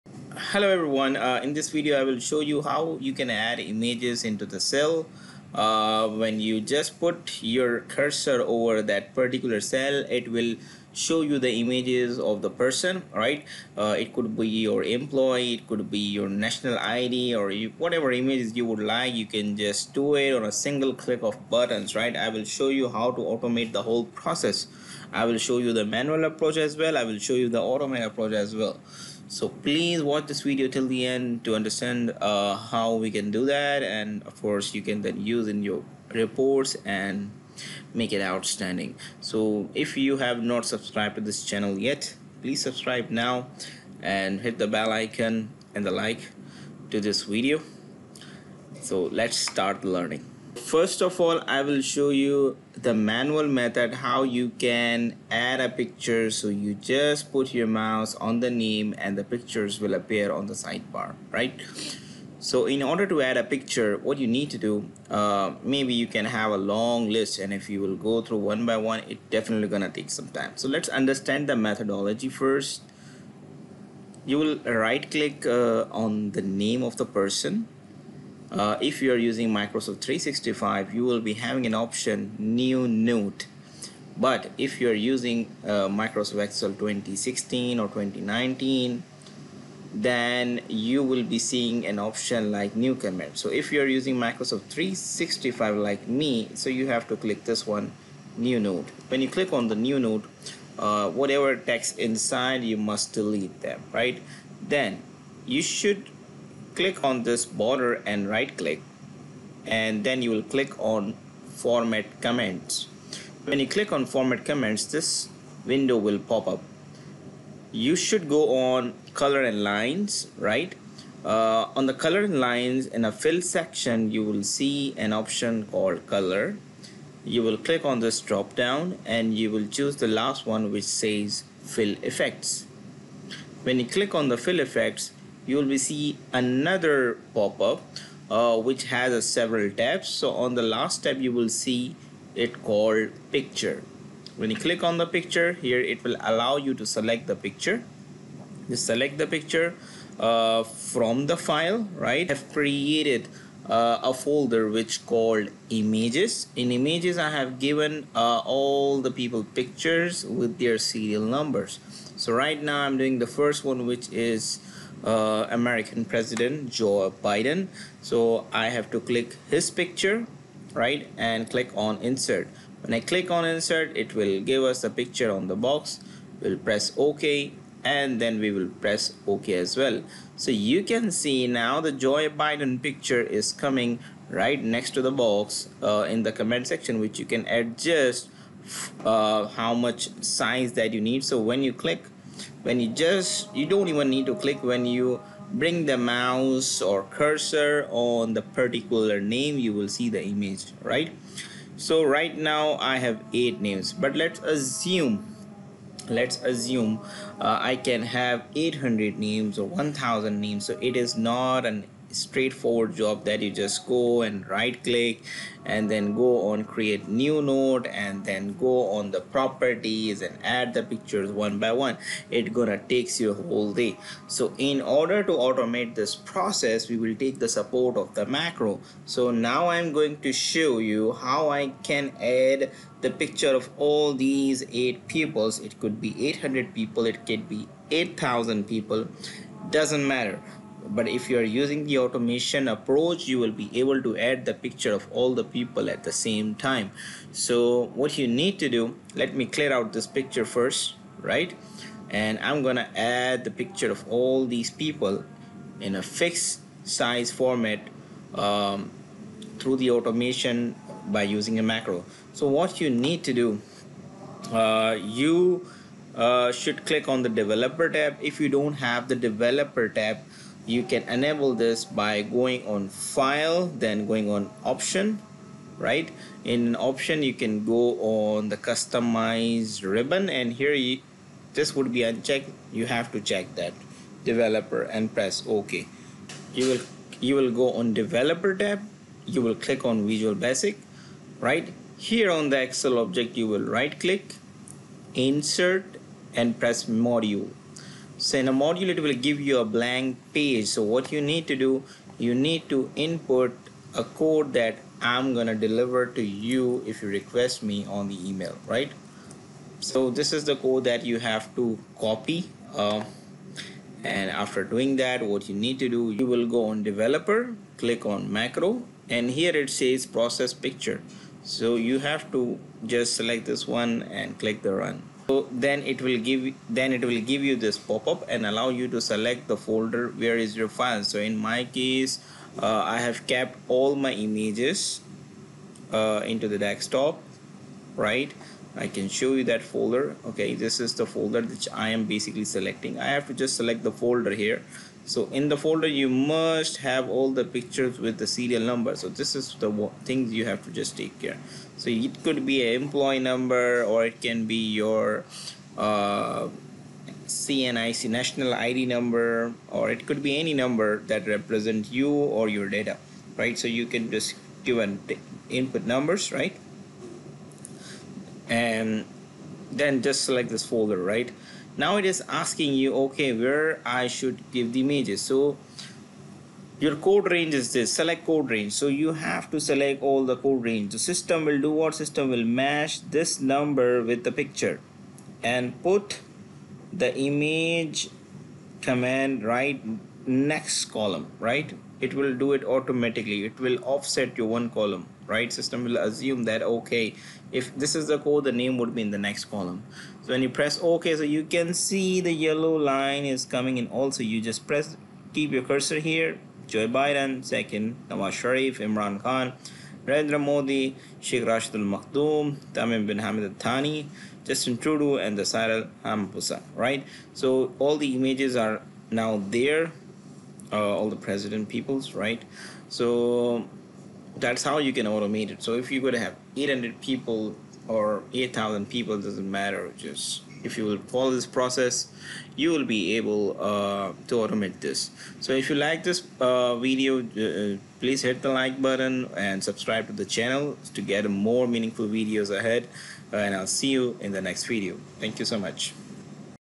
How to Add Image in Cell Comment Box in Microsoft Excel automatically for bulk data with VBA Macros
In this video you will learn the following:
How to Add image in Excel Comment Cell manually
How to Add Images in cell automatically in bulk.
How to automate adding images in comment box using Excel VBA Macros.

This Video is essential to watch for every person including students, officers and business person.

To get this file.